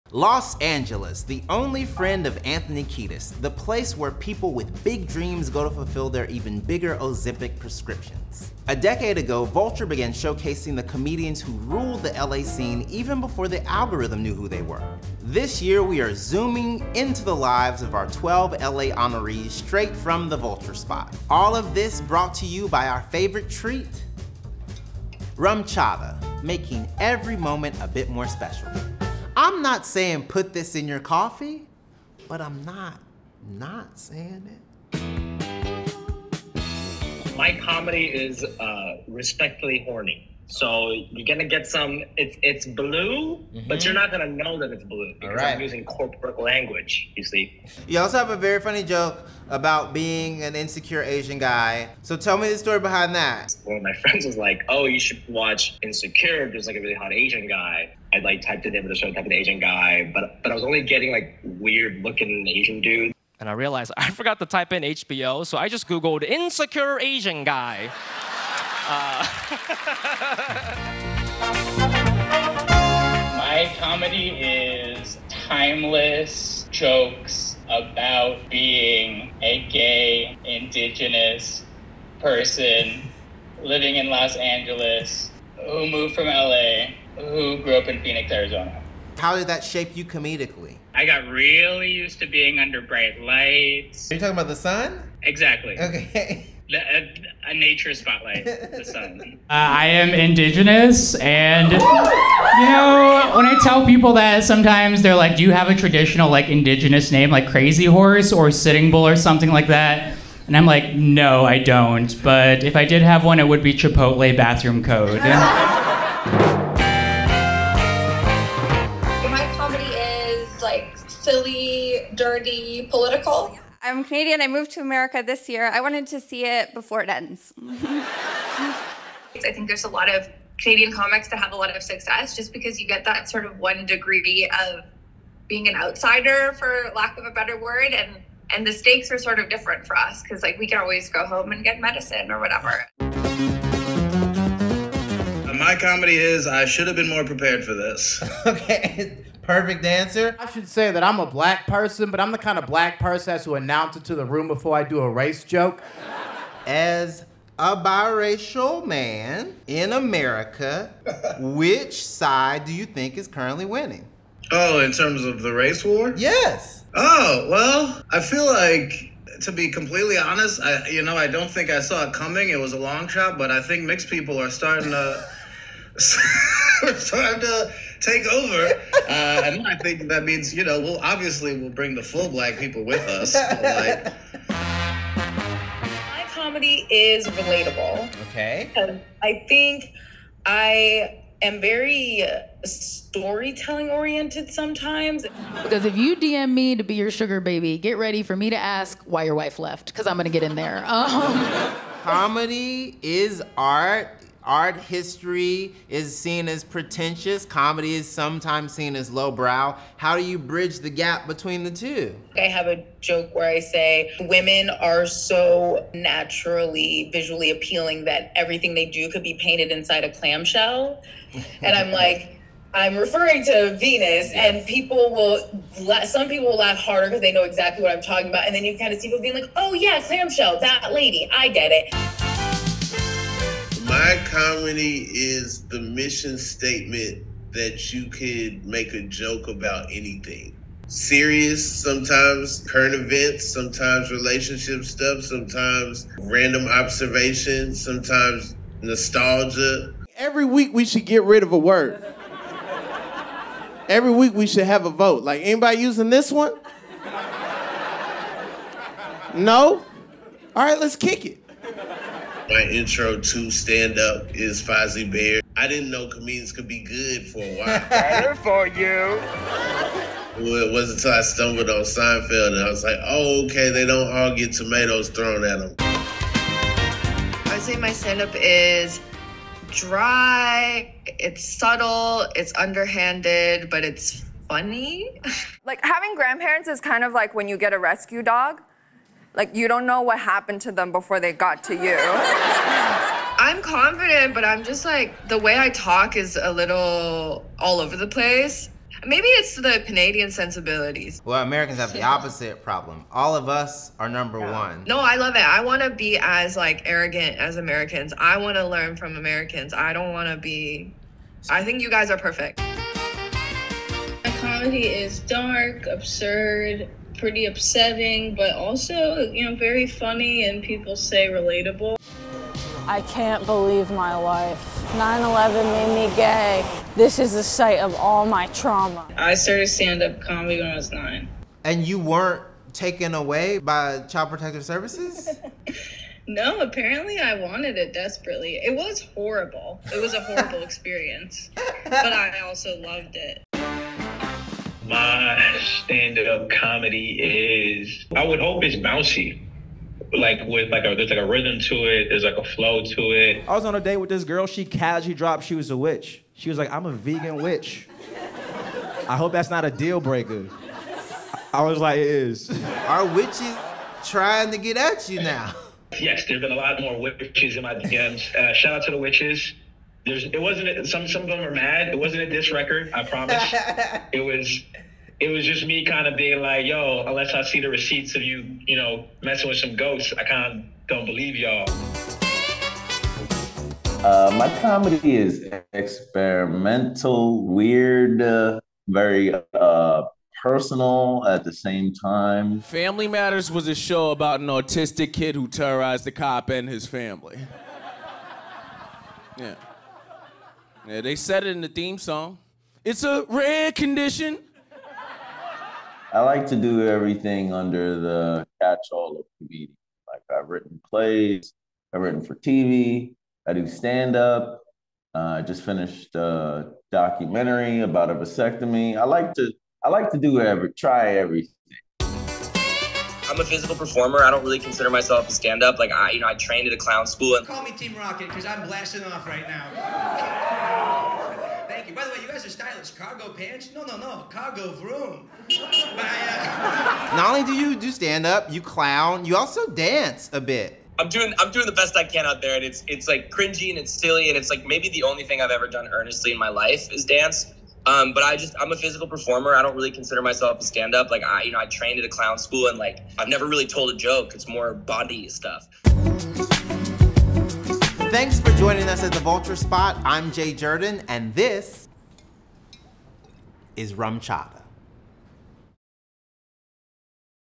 Get a Look at the Comedians You Should and Will Know (LA Version)
This year, Vulture’s list of “The Comedians You Should and Will Know” is going visual, with each of our honorees getting a chance to sit down with our host, comedian and list alumnus Jay Jurden, for a quick chat about their comedic sensibilities, funniest jokes, and unlikely origin stories. In this video featuring comedians based in Los Angeles, we learn what it’s like to start comedy at nine years old, how to perform comedy that is “respectfully horny,” and who is winning the ongoing race war. Watch it in full here, before we take a cue from some of the comedians on our list and chop it into 11 clips and strew them across social media. Presented by Rumchata!